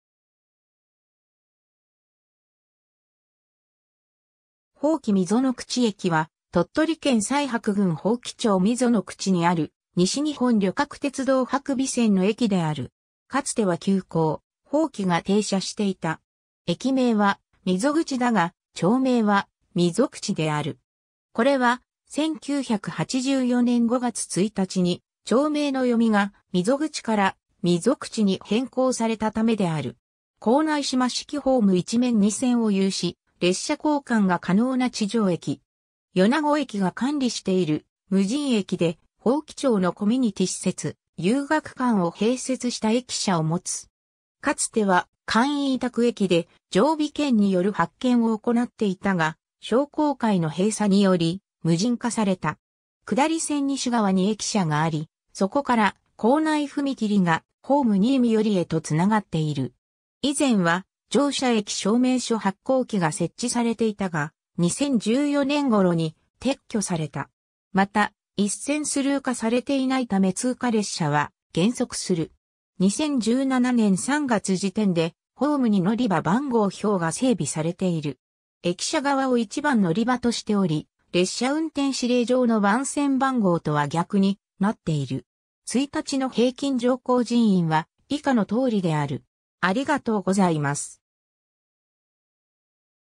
伯耆溝口駅
伯耆溝口駅（ほうきみぞぐちえき）は、鳥取県西伯郡伯耆町溝口（みぞくち）にある西日本旅客鉄道（JR西日本）伯備線の駅である。
かつては急行「伯耆」が停車していた。
 駅名は「みぞぐち」だが、町名は「みぞくち」である。
これは、1984年（昭和59年）5月1日に町名の読みが「みぞぐち」から「みぞくち」に変更されたためである。
 構内（2006年11月3日） 島式ホーム1面2線を有し、列車交換が可能な地上駅。
米子駅が管理している無人駅で、伯耆町のコミュニティ施設「遊学館」を併設した駅舎を持つ。
かつては簡易委託駅で、常備券による発券を行っていたが、商工会の閉鎖(正式には合併による閉鎖(移転先は岸本駅構内))により、無人化された。
下り線西側に駅舎があり、そこから構内踏切がホーム新見寄りへと繋がっている。
以前は、乗車駅証明書発行機が設置されていたが、2014年頃に撤去された。
また、一線スルー化されていないため通過列車は減速する。
 ※2017年3月時点でホームにのりば番号標が整備されている。
駅舎側（下り）を1番のりばとしており、列車運転指令上の番線番号（上りが1番線）とは逆になっている。
 1日の平均乗降人員は以下の通りである。